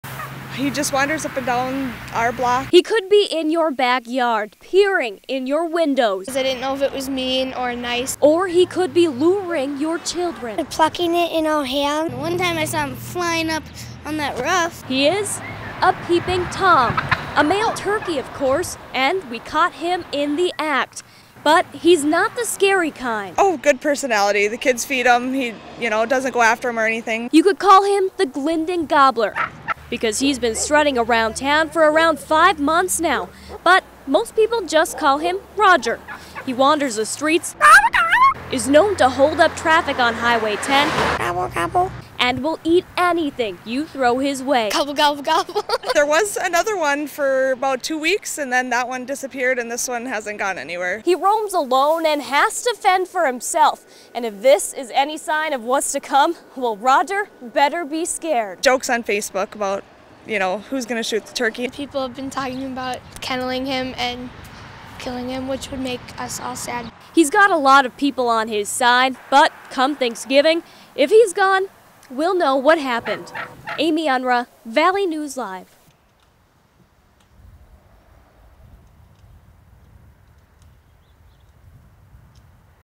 Peeping Tom Turkey
There's something strange in a valley neighborhood... lurking around people's yards, gobbling and looking in their windows. Valley News Team's Amy Unrau shows us what's strutting around Glyndon, Minnesota.

Karen Barbee of Glyndon says, "He just wanders up and down our block."

He could be in your back yard... peering in your windows.

Hailey Barbee of Glyndon says, "Didn't know if it was mean or nice or it would hurt anybody."

Or he could be luring your kids.

Garrett Barbee of Glyndon says, "He was plucking it in our hands."

"One time I saw him fly up on the roof," says Drake Barbee of Glyndon.

He is a peeping Tom! A male turkey, of course. And we caught him in the act. But he's not the scary kind.

Karen says, "Good personality. The kids feed him. He doesn't go after them or anything."

You could call him the Glyndon Gobbler, because he's been strutting around town for around five months, but most people just call him Roger. He wanders the streets, is known to hold up traffic on Highway 10 and will eat anything you throw his way.

Karen says, "There was another one for a bit, then it disappeared. This one hasn't gone anywhere."

He roams alone and has to fend for himself. And if this is a sign of what's to come... Then Roger better watch out!

Karen says, "Jokes on facebook about who's going to shoot him."

"People have been talking about killing him, which would make us sad," says Hailey.

He's got a lot of people on his side. But come Thanksgiving, if he's gone, we'll know what happened.

If you want to catch a peek at Roger the next time you're in the area, he hangs out just off Highway 10, fittingly, few blocks west of Partridge Avenue.